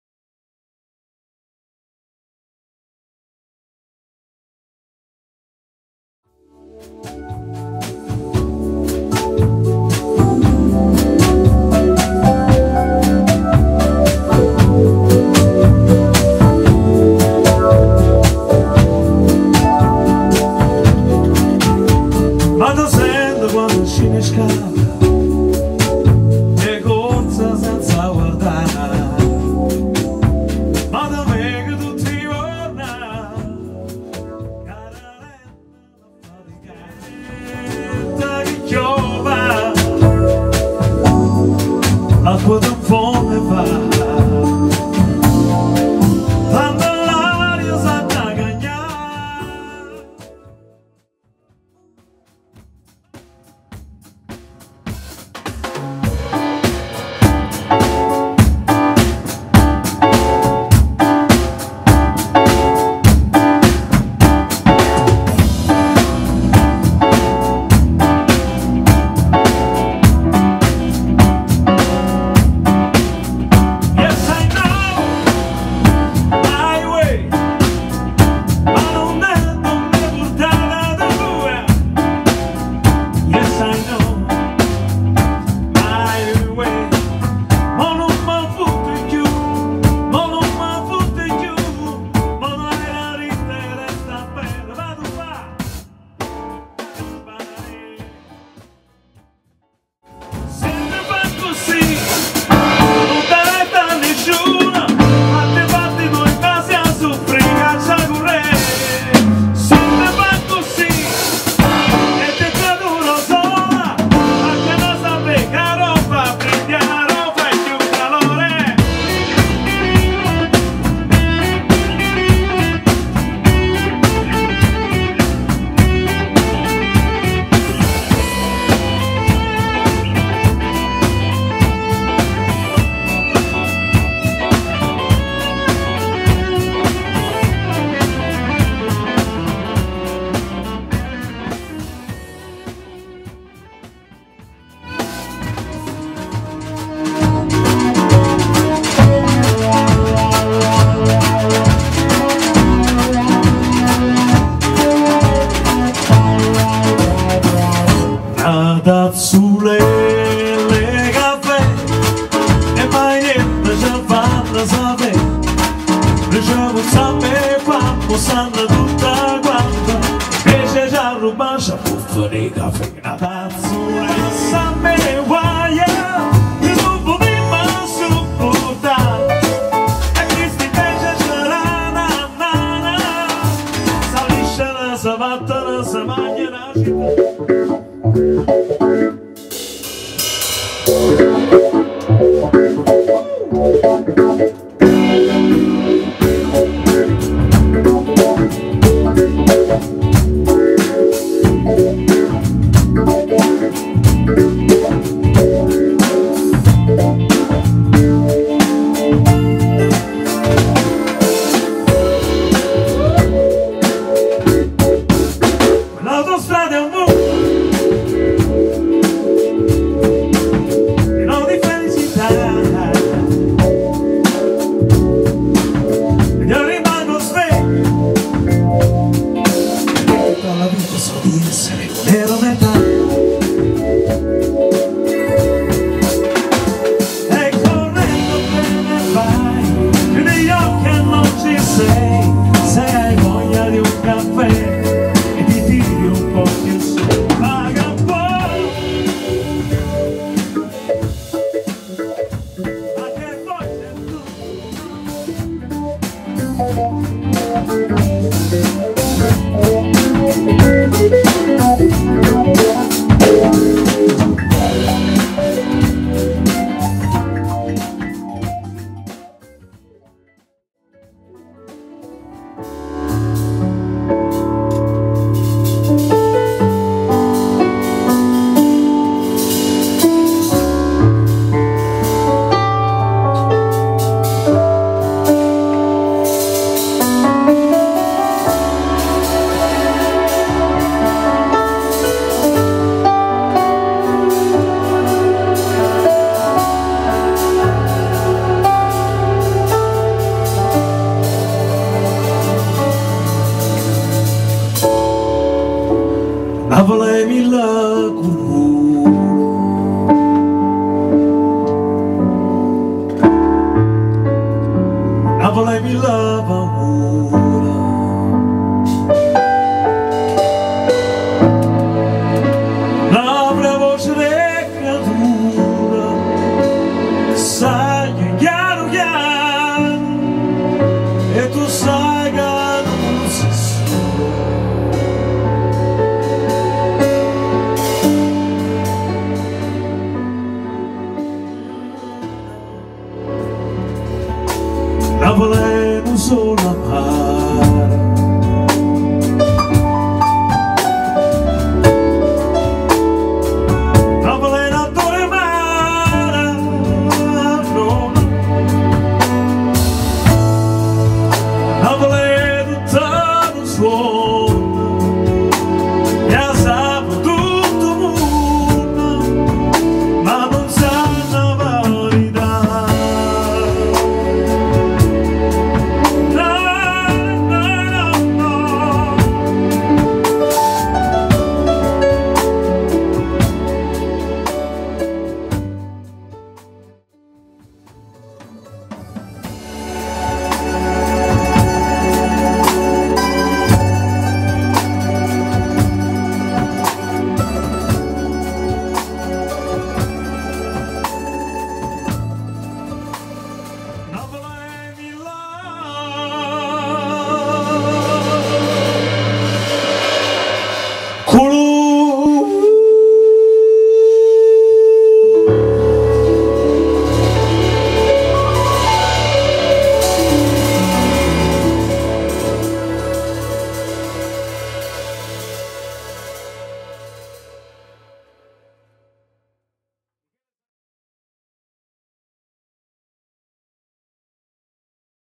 VITO CANTA......PINO. DEMO
VITO CANTA. PINO. " VIDEO DEMO". VITO BARBATO tastiera e voce/ MICHELE SANTIGLIA batteria. "TRIBUTO A PINO DANIELE."